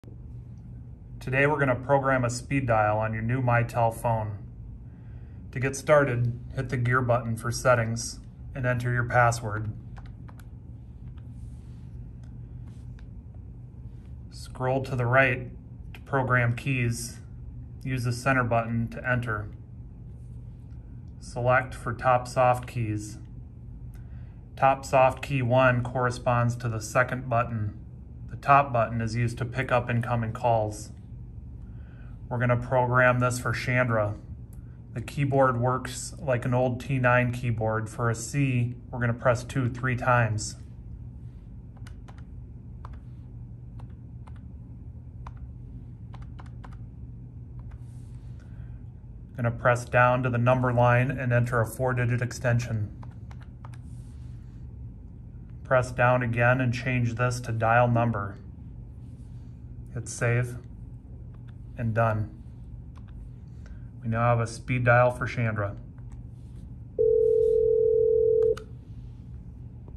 Speed Dial on Mitel 6930
How to setup speed dial on a MItel 6930 on Mitel Connect on premise,